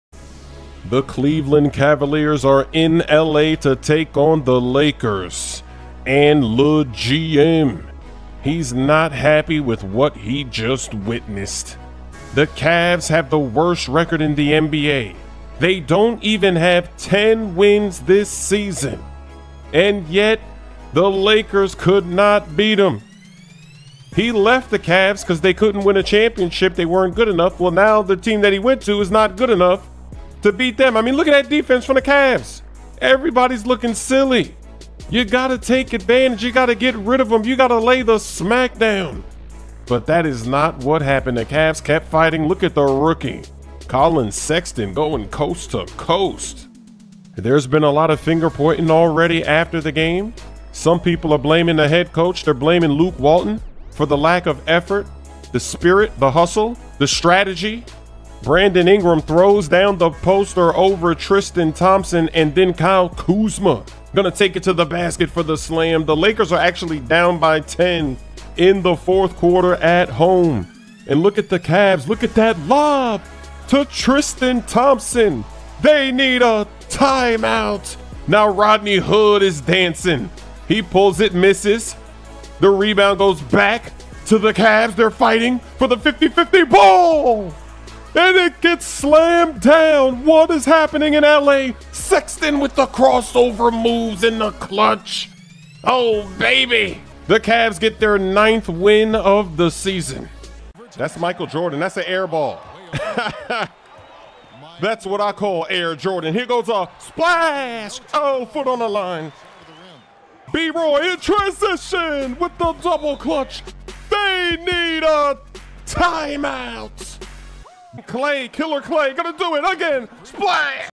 【外國NBA評論】騎士 復仇成功？湖人 輸給聯盟最差的 騎士！ 勒總管 詹姆斯 很不高興！湖人 vs 騎士 2018 19 NBA （ChrisSmoove中文）
騎士在洛杉磯對上湖人
然後勒總管LeGM
他對於他看到的畫面很不高興
騎士擁有聯盟最爛的記錄
他們這季連10勝也沒有
但是湖人卻不能打敗他們
他（詹姆斯）離開騎士是因爲他們不能贏總冠軍
隊伍不夠強
但是現在他前往的球隊卻不能打敗他們（騎士）
你看看這騎士的防守
每個人看起來都像傻瓜
你要占領優勢 你要撇除他們（騎士）
你要狠狠擊敗他們
但這不是實際發生情況
騎士繼續爭鬥著
看看新秀Sexton從東岸到西岸
在比賽過後很多人被指責
有些人在責備總教練 他們責怪Walton
因爲他不夠努力 缺乏氣勢 不拼命 缺少策略
英格拉姆在湯普森丟下一張海報
然後庫子馬會切入籃筐灌籃
湖人其實在第四節在主場落後10分
然後看看騎士 看看這空中接力！
傳給湯普森！他們需要叫暫停
現在胡德在跳舞 他投籃打鐵
籃板回到騎士手中
他們爲了五五開的球在奮鬥著
還被灌籃 洛杉磯發生了什麽事
Sexton在關鍵時刻一個花式運球
騎士拿下本季第九場勝利
騎士 復仇成功？湖人 輸給聯盟最差的 騎士！ 勒總管 詹姆斯 很不高興！湖人 vs 騎士